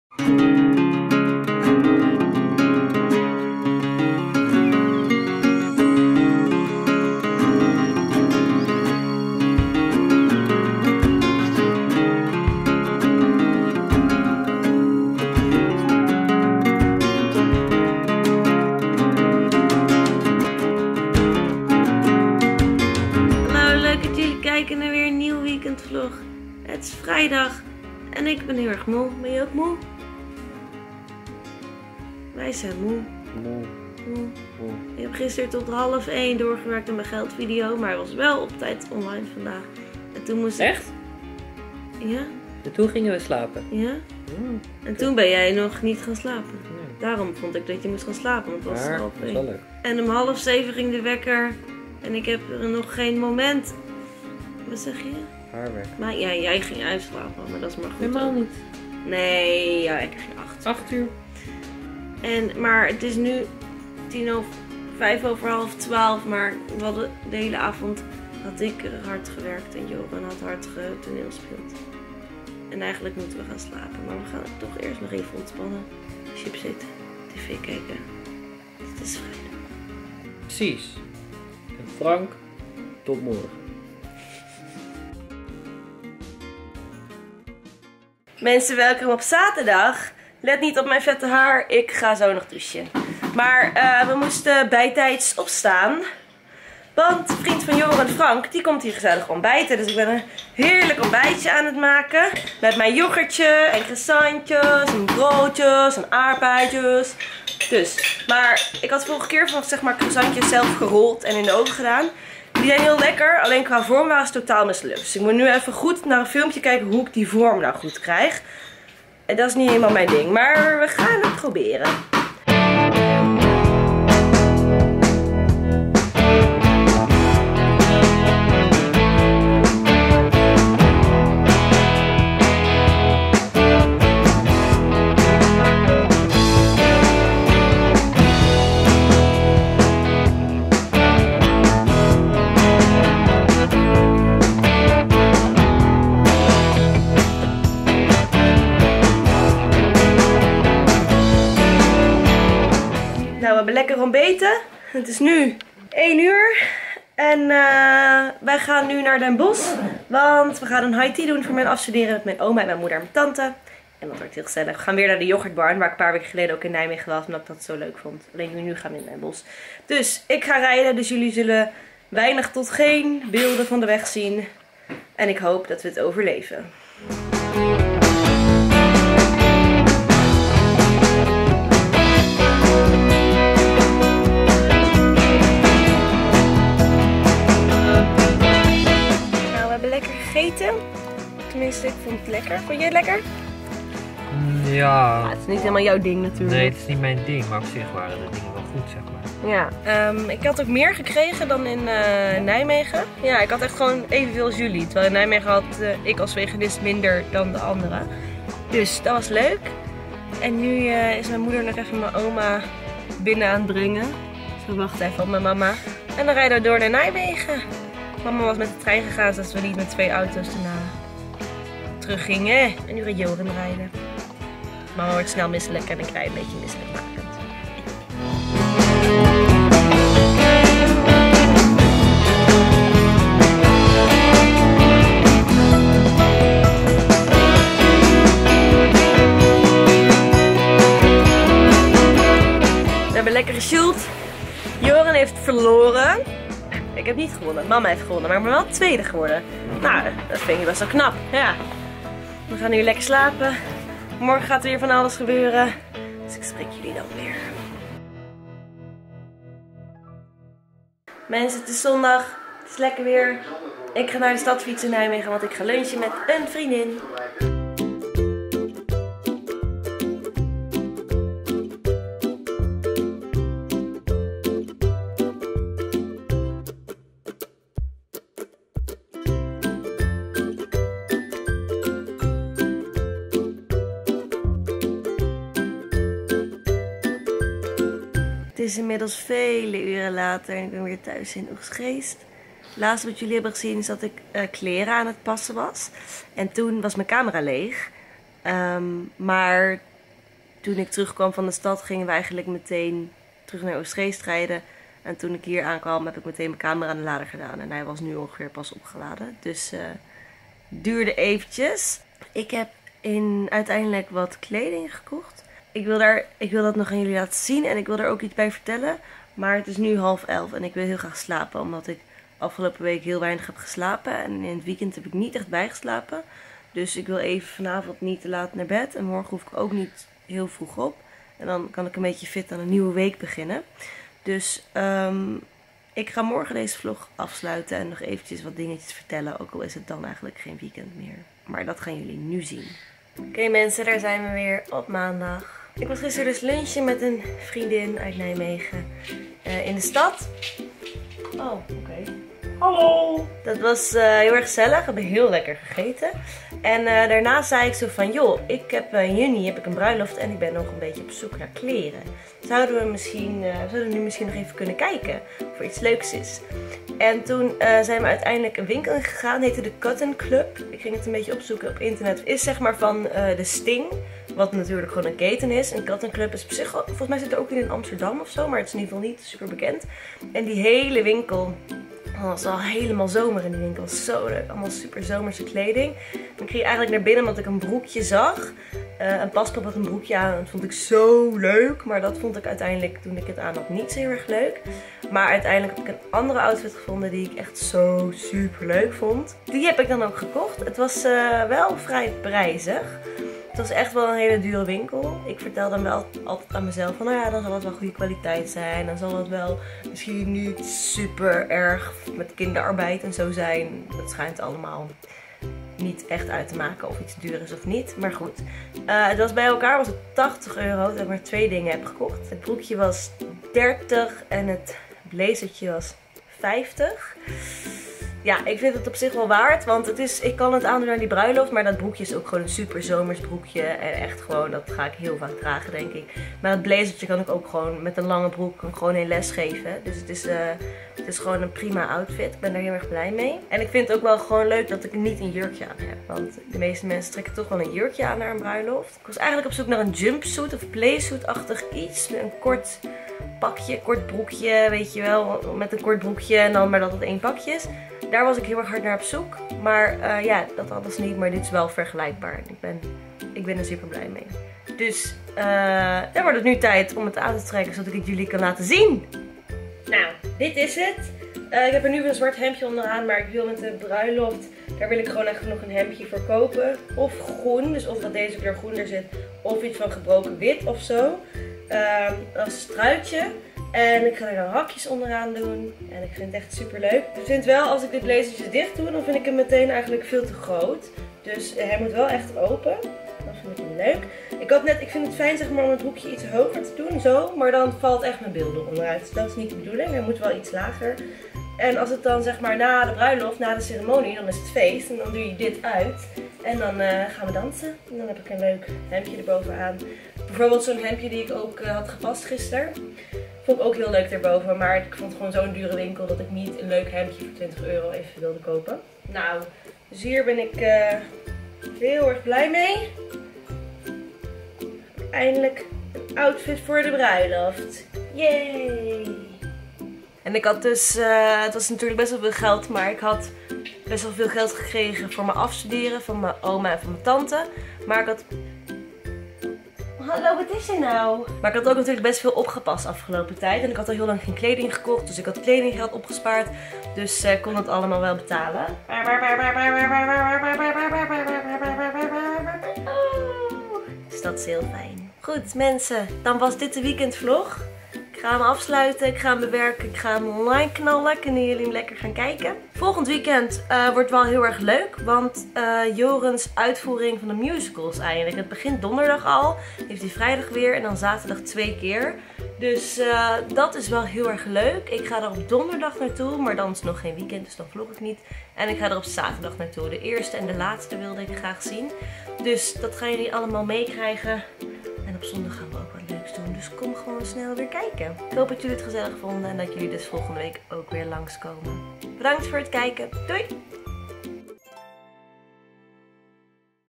EINDELIJK EEN BRUILOFT-OUTFIT / Weekendvlog #46
In deze weekenvlog ontbijten we uitgebreid in Oegstgeest, hebben we een high tea in Den Bosch en ga ik lunchen in Nijmegen!

Met de geweldige muziek van
Freedom Trail Studio
Wayne Jones